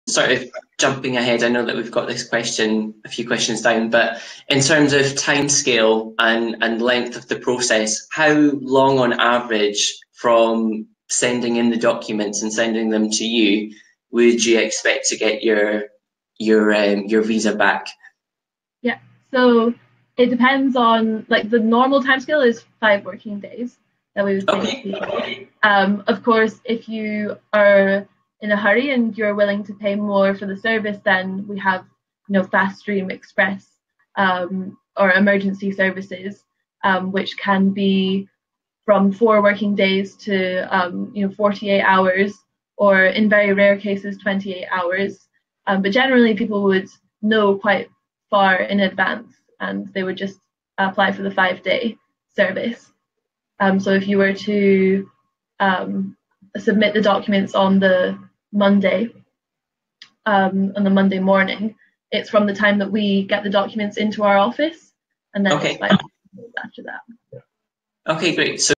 How Long Does It Take To Process? | TEFL Org
We interviewed Joey and Abi from i2Visa to ask about the process of applying for a Chinese visa.